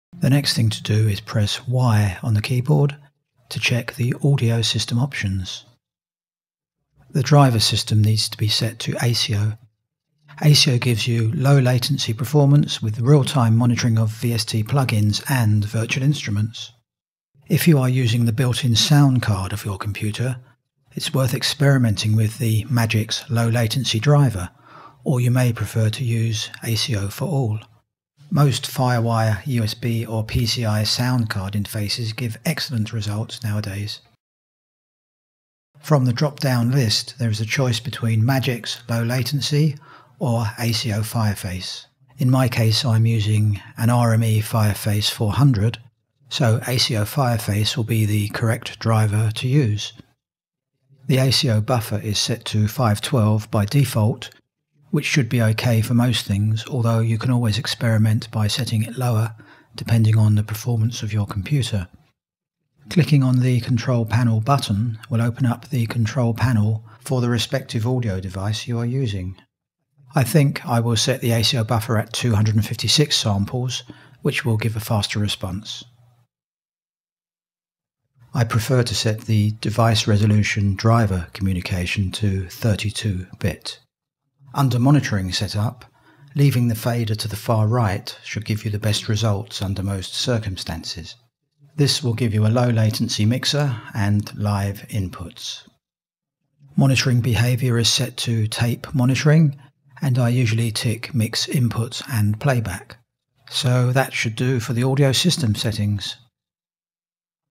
Samplitude Basics 04:Audio System Options.
How to set up the audio options in Samplitude or Sequoia. This includes choosing the Asio soundcard driver.